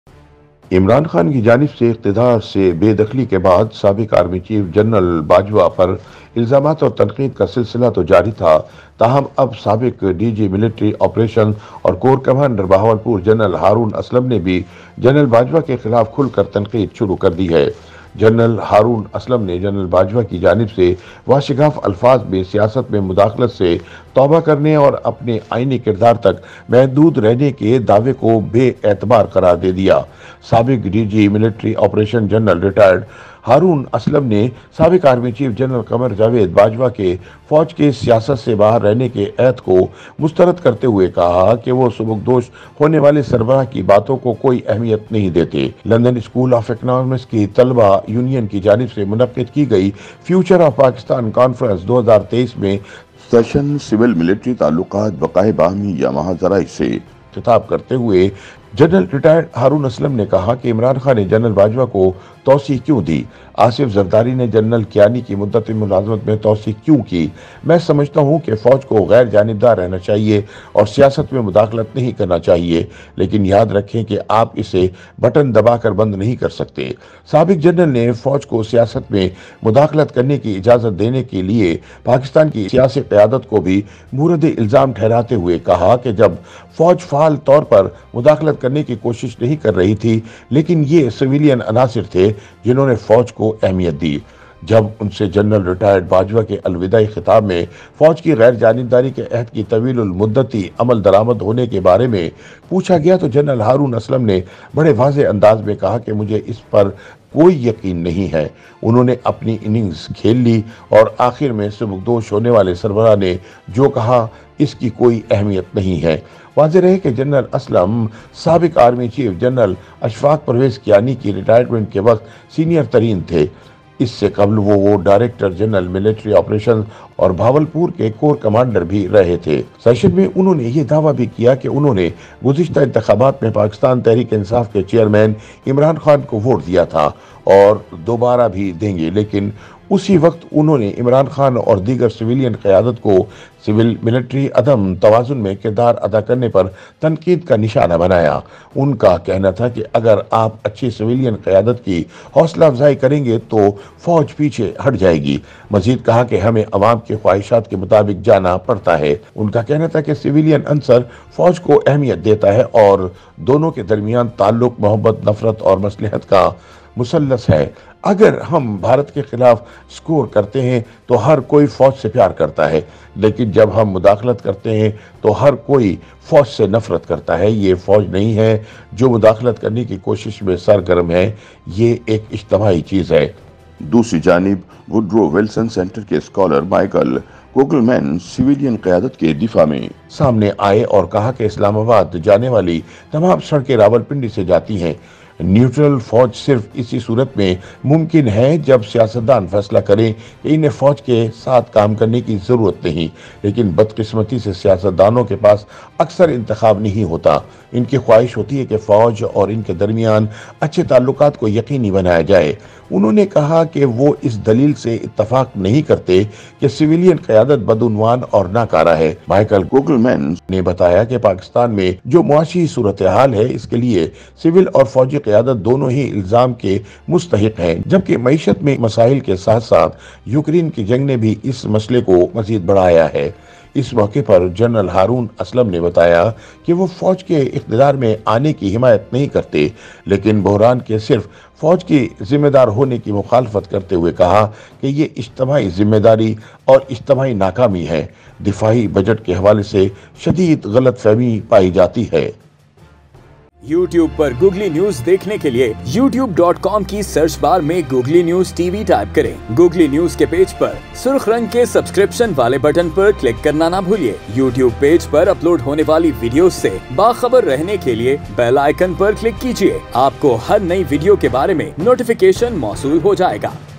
Gen Haroon Aslam Nai Gen Bajwa Ko Na Qabilay Aitbar Kiyun Qarar Dai Dia? | Googly News TV
Gen Haroon Aslam Nai Gen Bajwa Ko Na Qabilay Aitbar Kiyun Qarar Dai Dia?